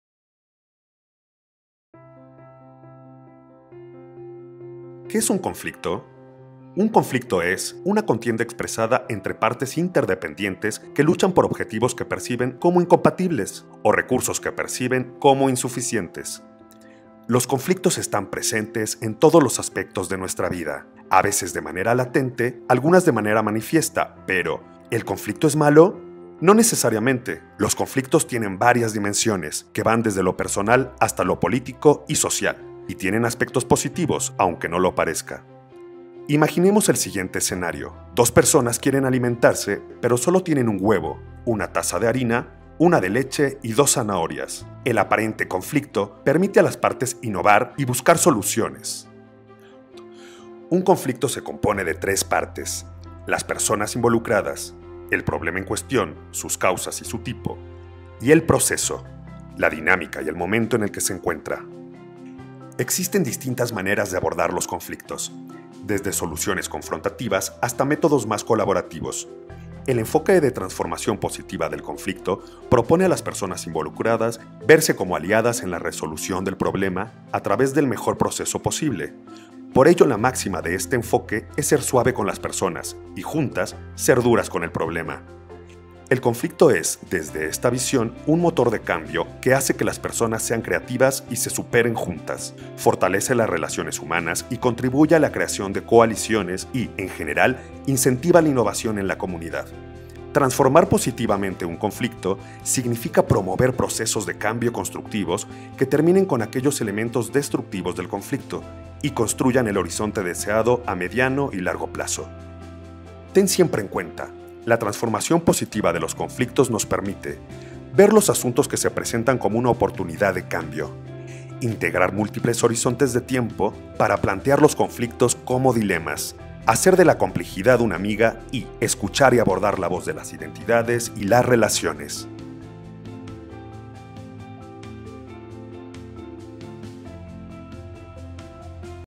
Transformación positiva de conflictos
Los conflictos no se crean ni se destruyen, solo se transforman. Las personas involucradas pueden ser aliadas en la resolución de un problema. Aprovecha los conflictos como oportunidades positivas para el cambio.